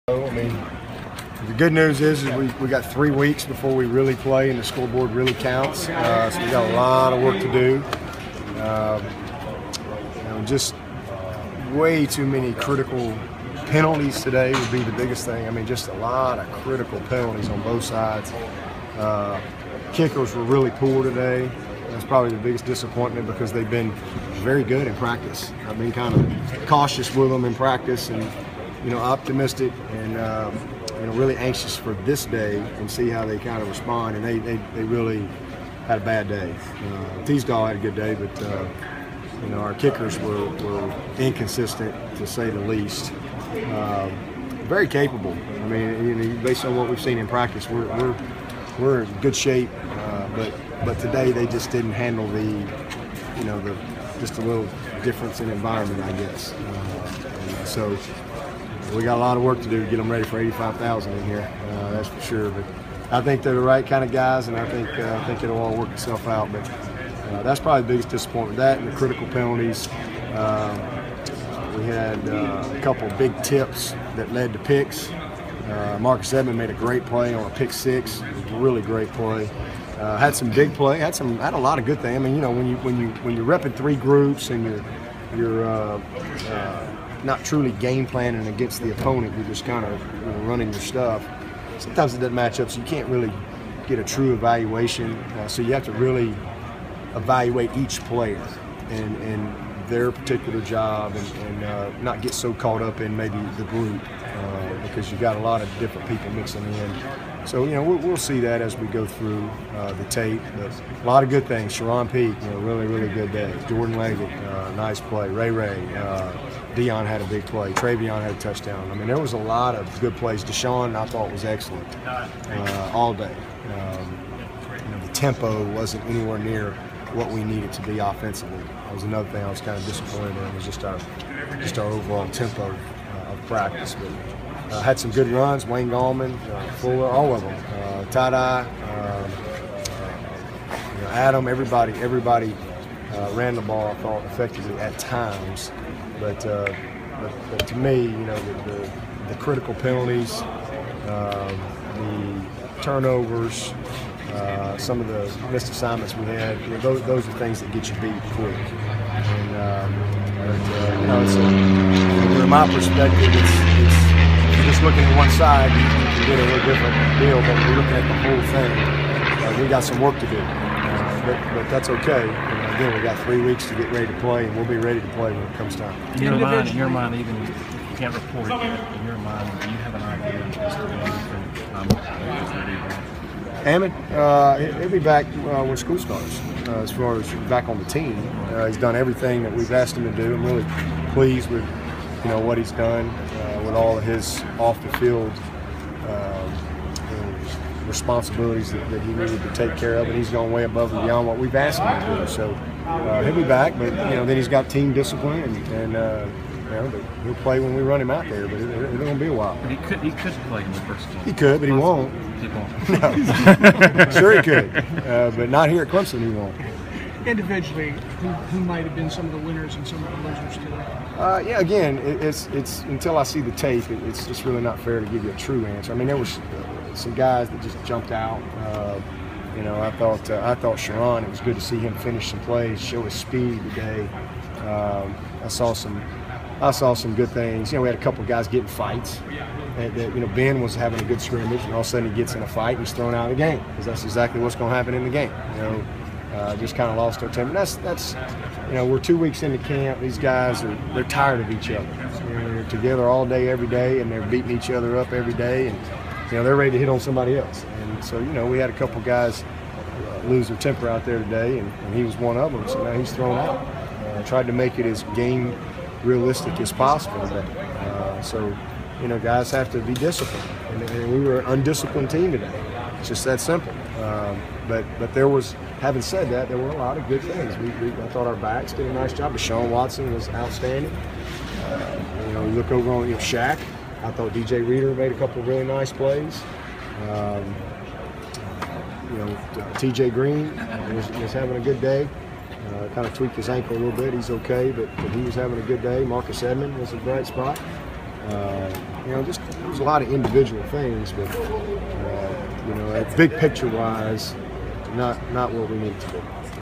Tigers see things to work on in scrimmage
Clemson coach wraps the first stadium scrimmage of the fall camp.